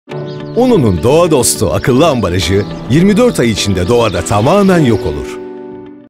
Uno'nun doğa dostu akıllı ambalajı ile TAM BESLEN, DÜNYAYI KORU!
Katkı ve koruyucu içermeyen Uno Premium Çok Tahıllı ve Siyez Buğdaylı, Premium Çavdar ve Karabuğdaylı ve Denge Tambuğdaylı ve Çiya Tohumlu ekmekler «DOĞA DOSTU AKILLI AMBALAJLARINDA»